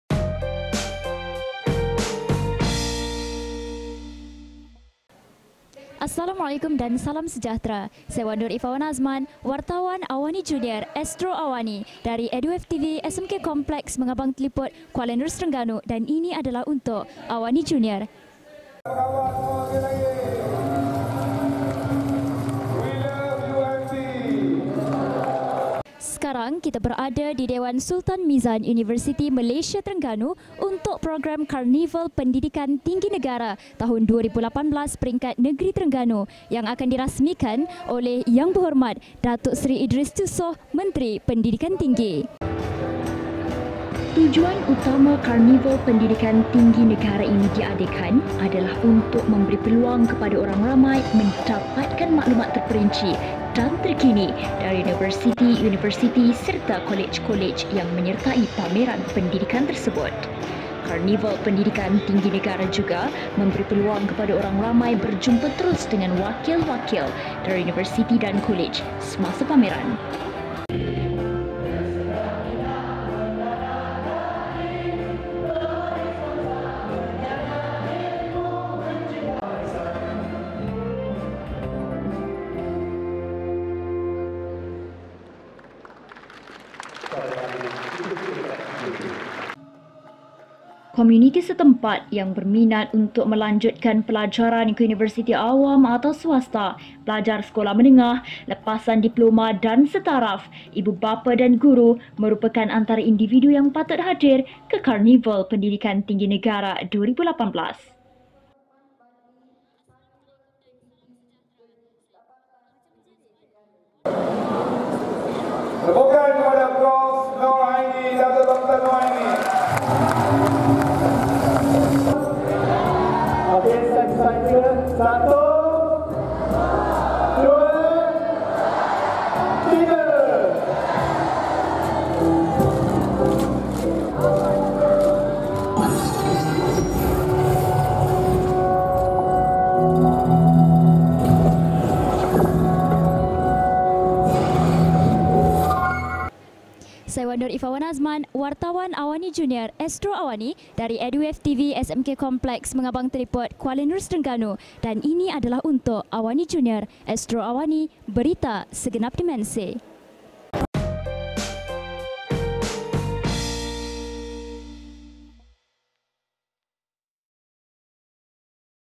AWANIJr: YB. Dato' Idris Jusoh di Karnival Pendidikan Tinggi , UMT
Adik Wan Nur Iffah Najihah Wan Azman (18 tahun ) wartawan AWANIJr dari Eduwave TV ,SMK Kompleks Mengabang Telipot, Kuala Nerus, Terengganu berada di Dewan Sultan Mizan Zinal Abidin, UMT , Terengganu untuk sama-sama menyaksikan perasmian Karnival Pendidikan Tinggi Peringkat Negeri Terengganu oleh YB Dato’ Idris Jusoh, Menteri pendidikan Tinggi , Malaysia. Majlis berlangsung dengan meriah dan gemilang.